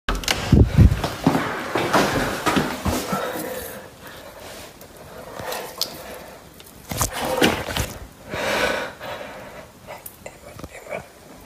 Nightmare Fuel: Snake Removed After Found in Woman's Toilet
THAT'S NOT HOW YOU SNAKE A DRAIN! A woman in Tuscon, Arizona, found a coachwhip snake nesting in her toilet after returning from vacation. A snake handler from Rattlesnake Solutions was able to wrangle the reptile and catch the encounter on camera. The snake was returned to the wild after being captured.